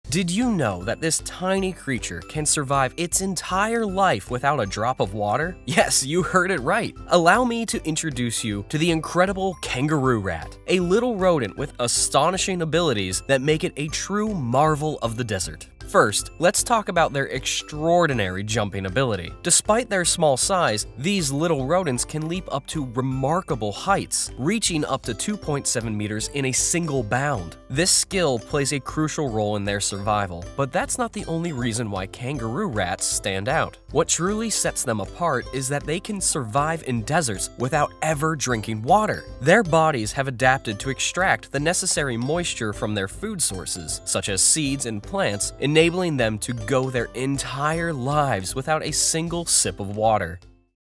Kangaroo Rat 🐭 Jumping for Survival!
Introducing the Kangaroo Rat, a fascinating desert-dwelling creature found in North America. Despite its name, this pint-sized rodent isn't related to kangaroos at all! Known for its incredible jumping abilities, the Kangaroo Rat uses its long hind legs to hop around, covering impressive distances in search of seeds and vegetation. With oversized hind feet and a distinctively long tail, it's a true desert acrobat, well-adapted to its arid habitat. Catch a glimpse of the Kangaroo Rat's unique features and survival strategies in this quick look!